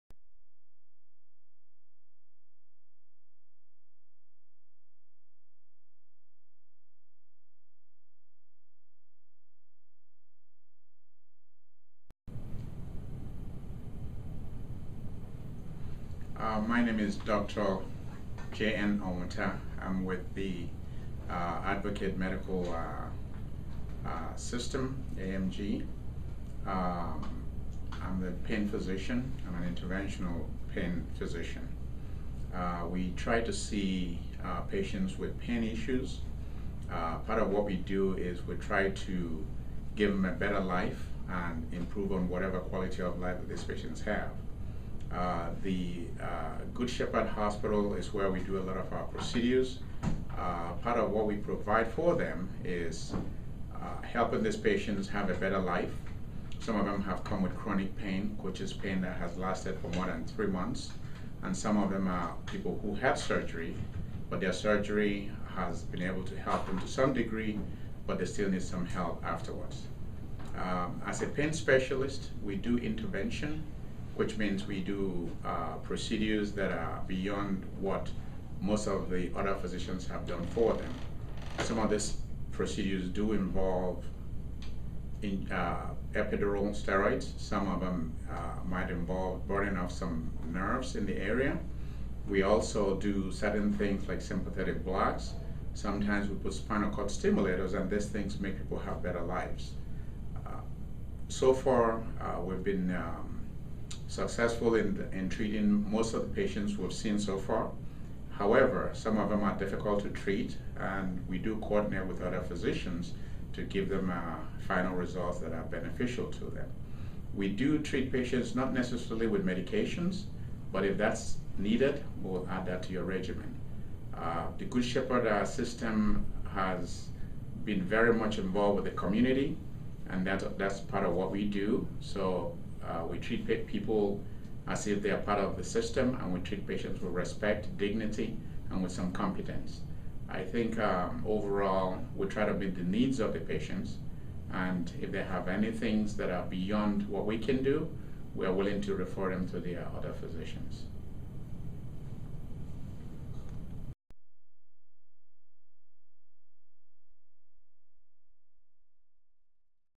Interventional Pain Management
Medical Director of our Interventional Pain Management Program discusses a variety of pain management treatments.